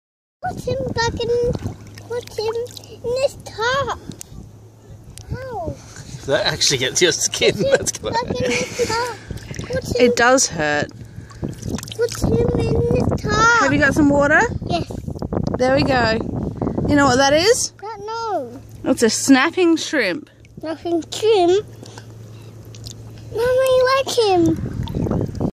Snapping Shrimp
Found some snapping shrimp down at the beach, this one obliged and did a few snaps for me on video.